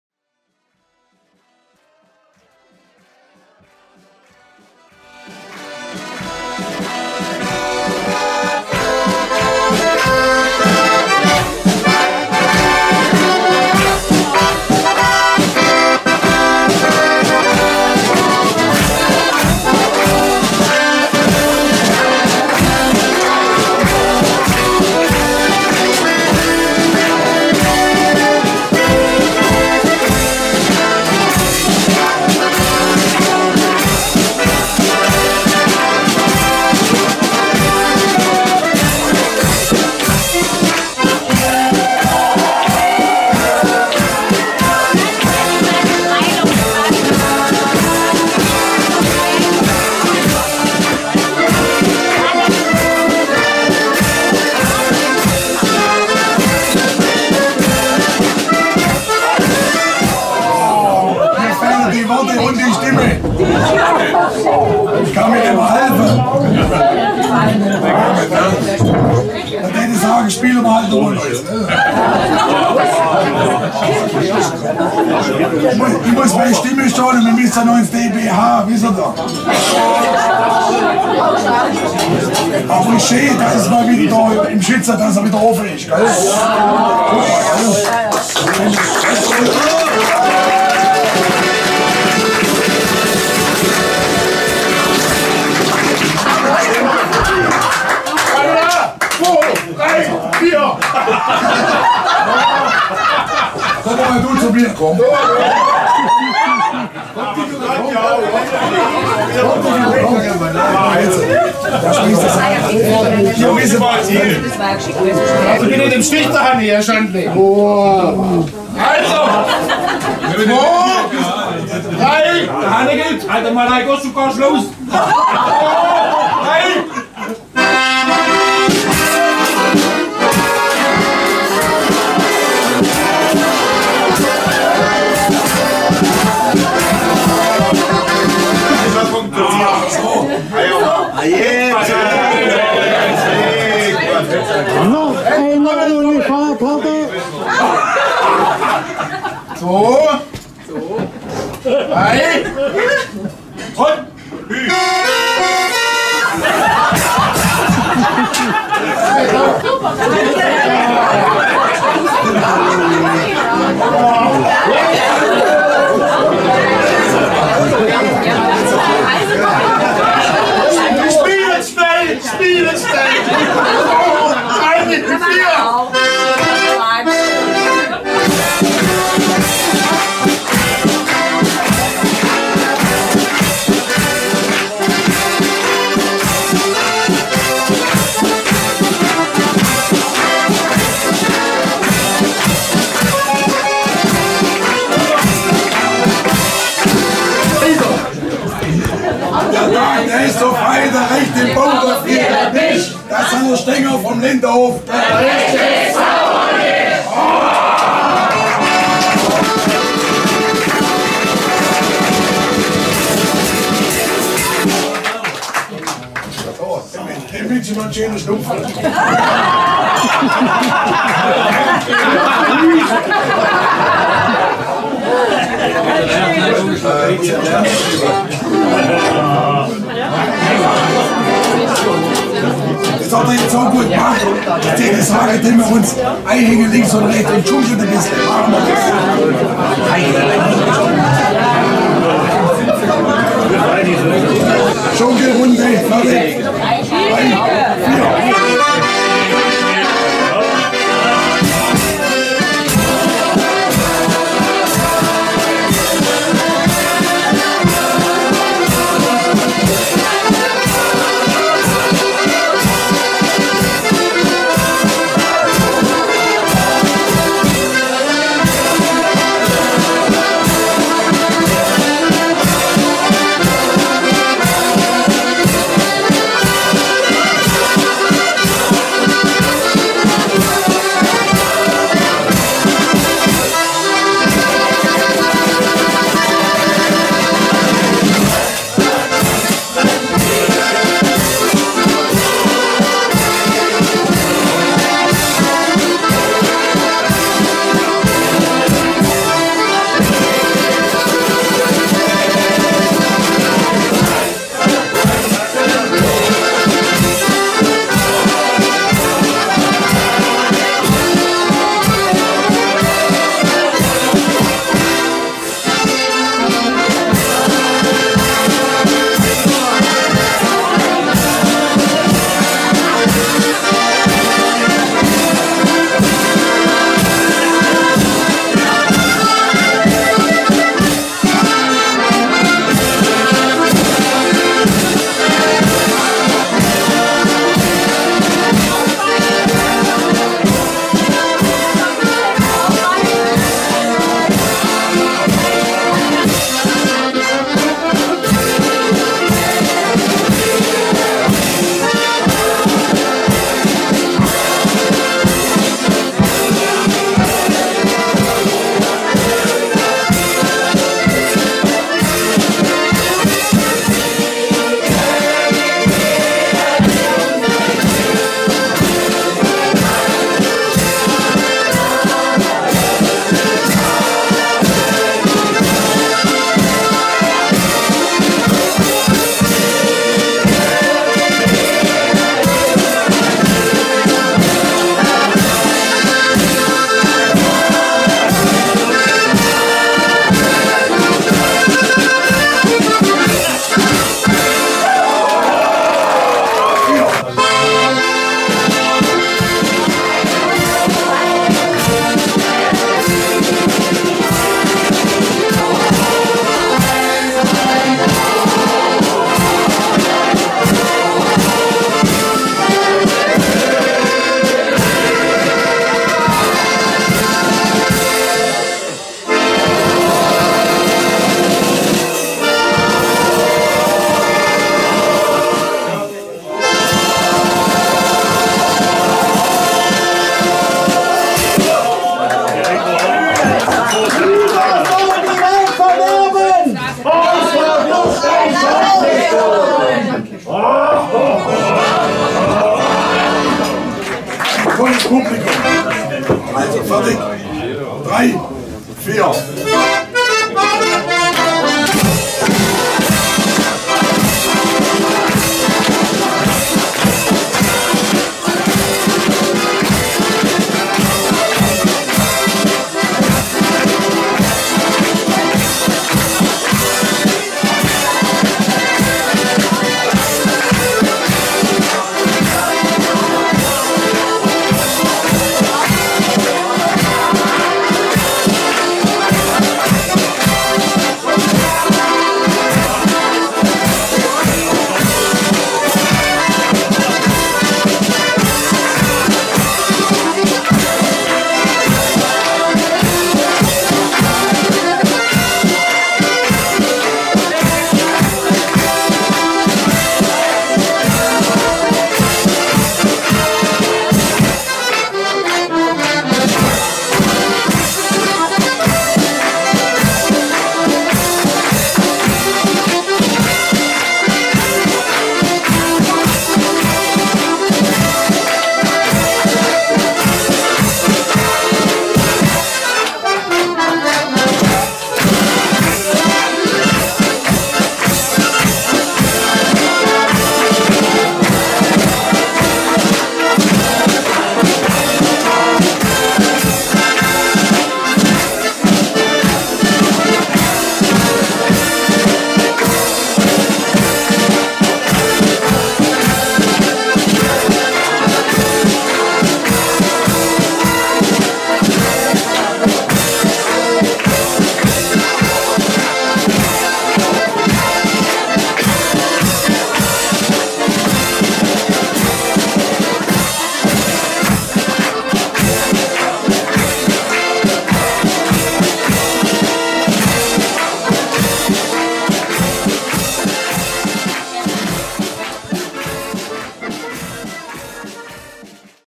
FASNET 2019 Oberndorf am Neckar - Schantlekapelle Lindenhof im Schützen
FASNET 2019 Oberndorf am Neckar - Schantlekapelle Lindenhof im Schützen, Dirigent: Lorenz Eisele